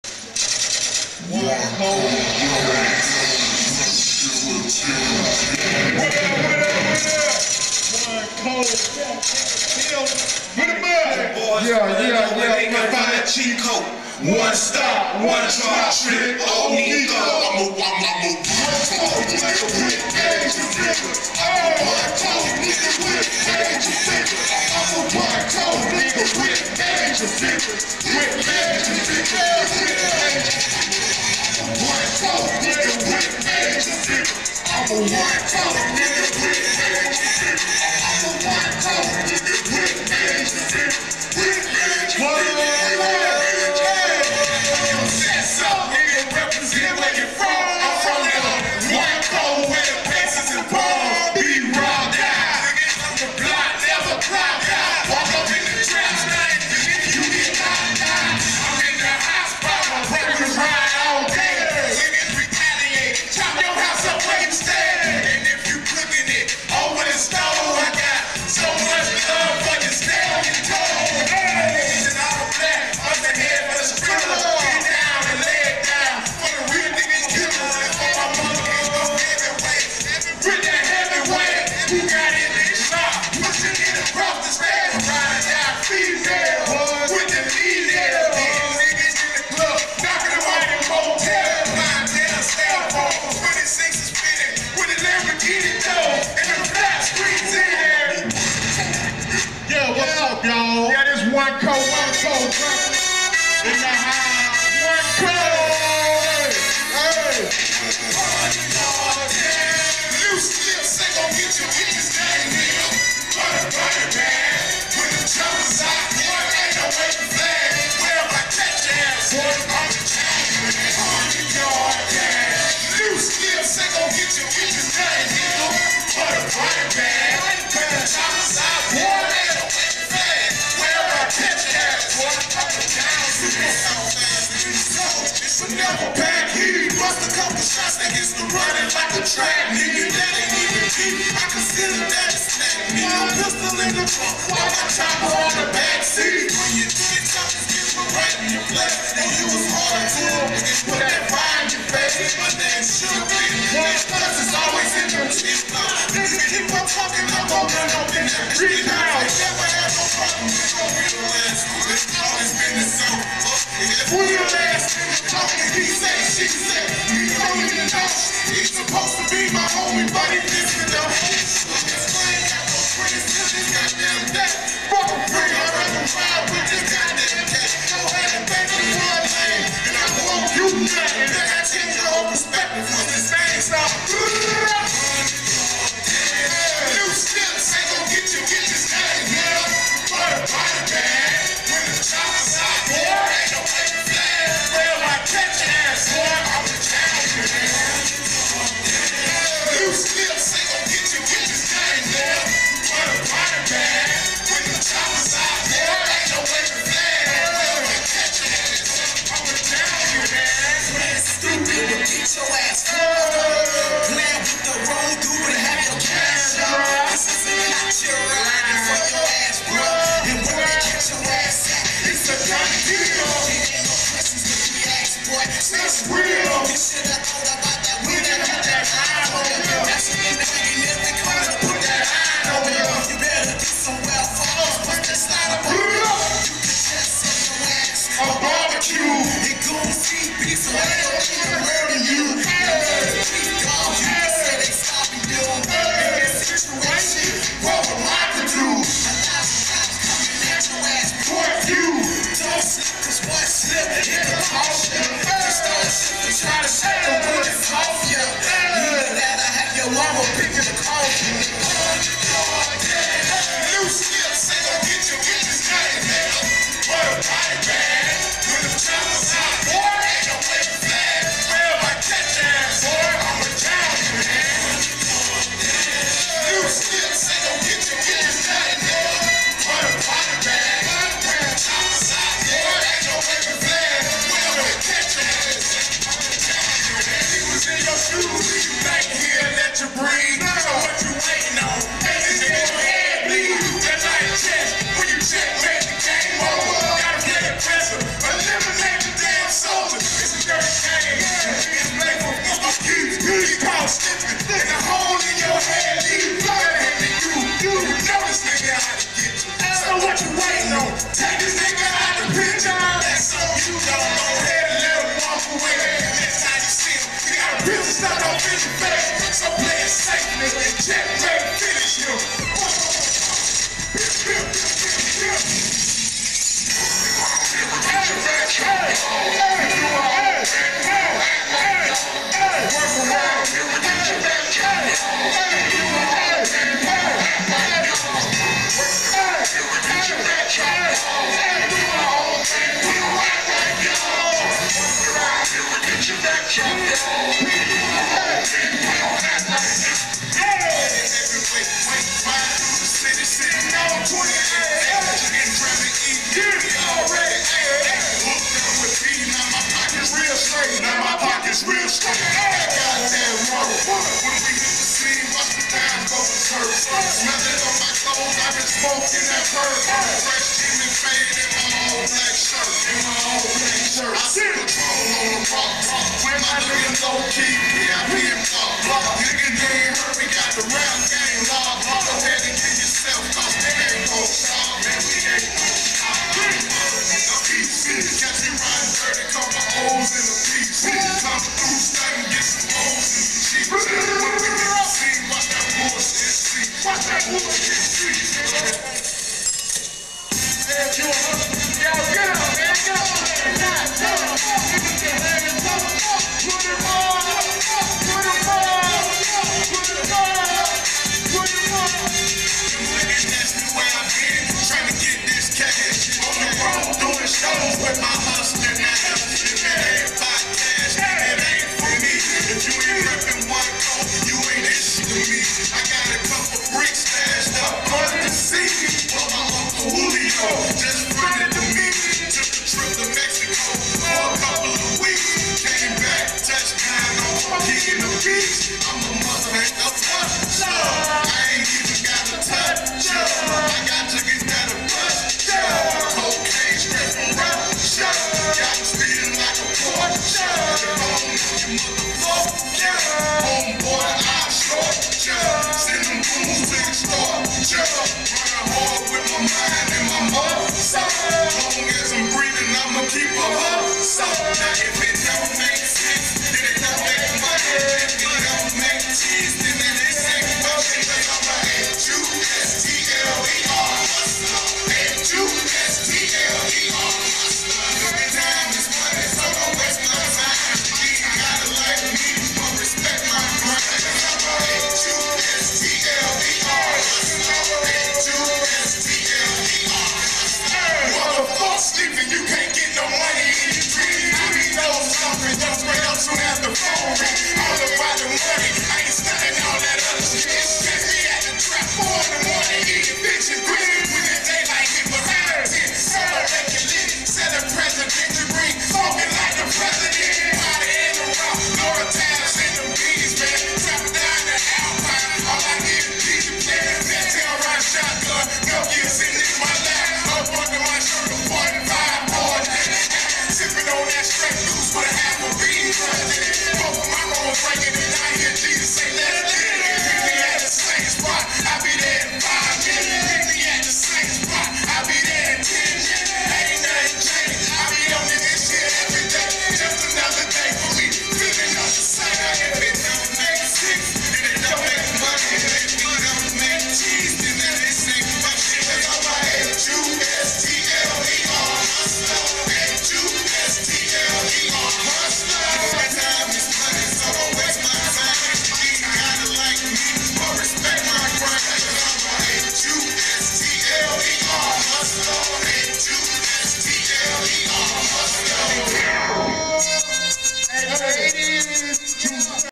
Manatee County Rap Fest
One Co Perform Live The Guillotine Lil One Block Pip Peeze